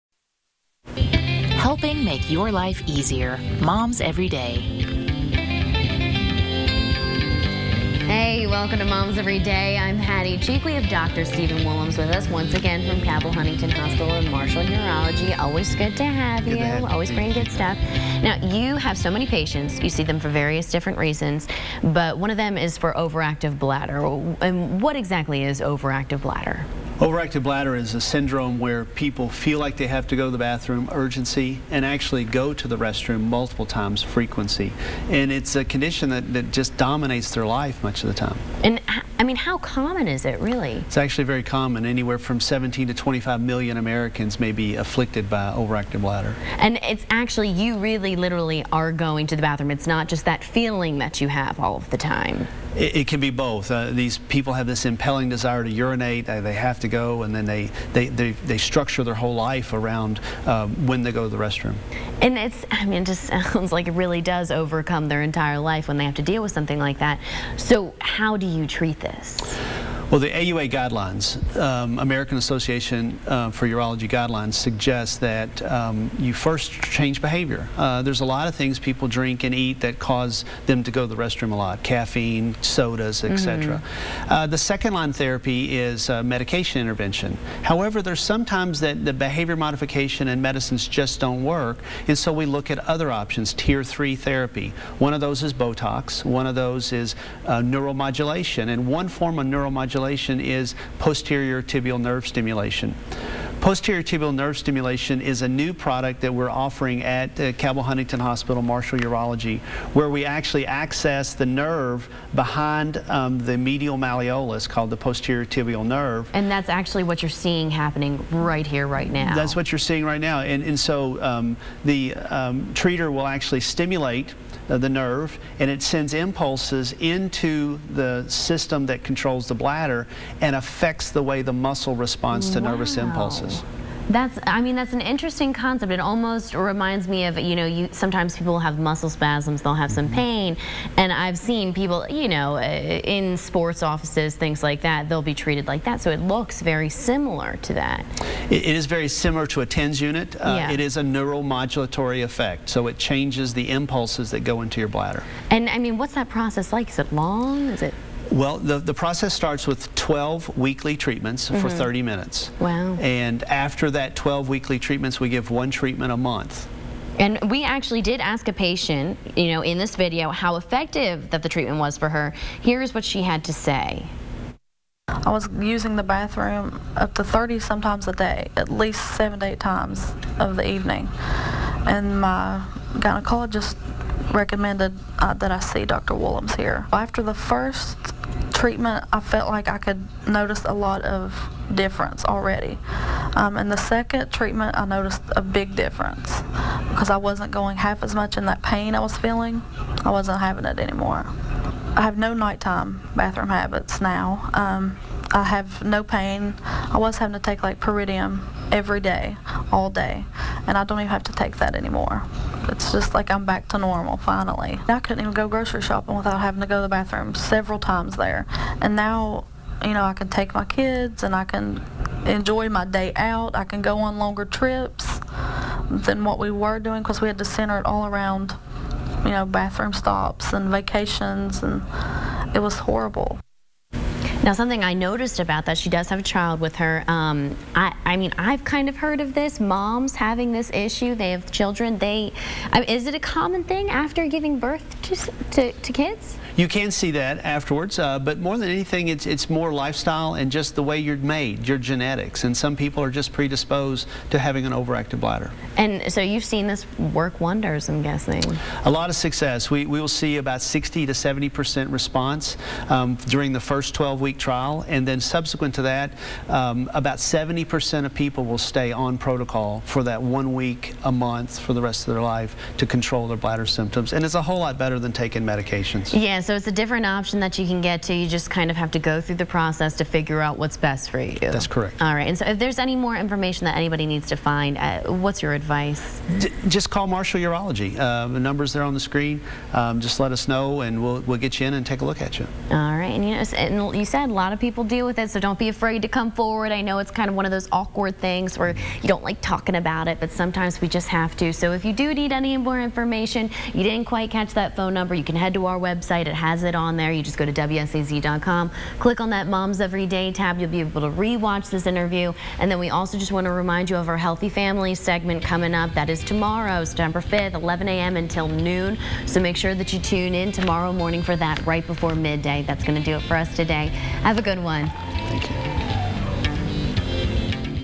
Overactive Bladder - Dr. Woolums
Dr. Stephen Woolums of Marshall Urology talks about the treatment and relief for those suffering from overactive bladder.